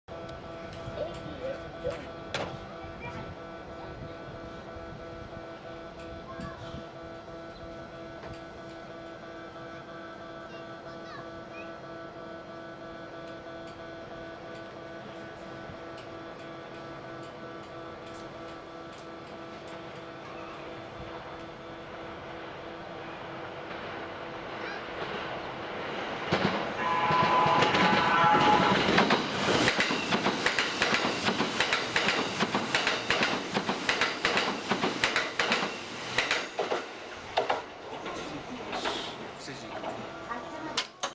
～桑名通過 警笛!!～ 賢島行き特急「しまかぜ」通過!! 近鉄50000系(しまかぜ)
桑名駅にて。